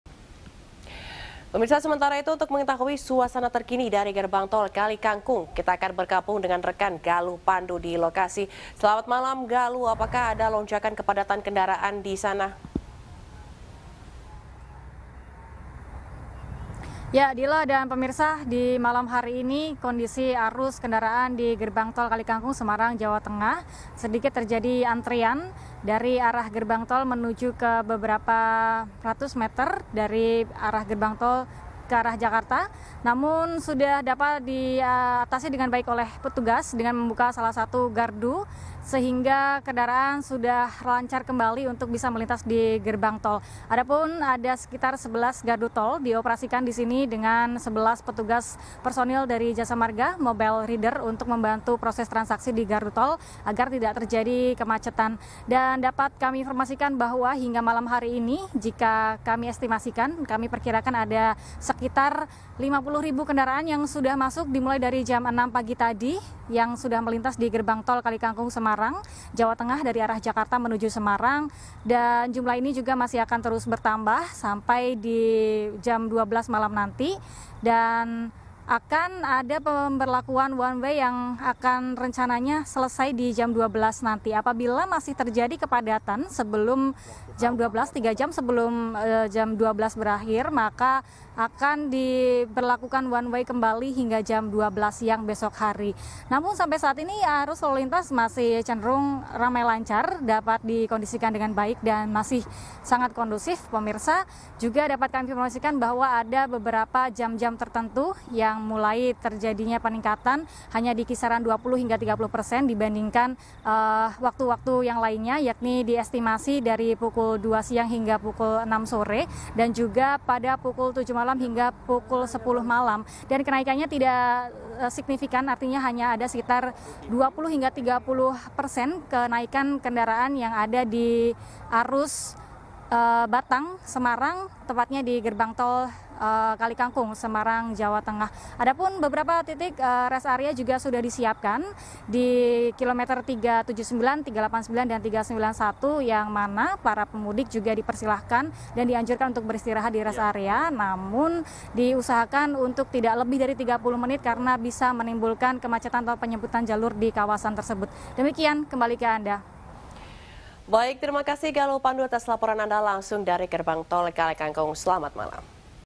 H-2 Lebaran, 50 Ribu Kendaraan Masuk Gerbang Tol Kalikangkung
Kendaraan pemudik terus memadati Gerbang Tol Kalikangkung, Semarang, Kamis (20/4/2023).  Pantauan di lokasi, gerimis sempat melanda gerbang tol yang berada di KM 414 ini.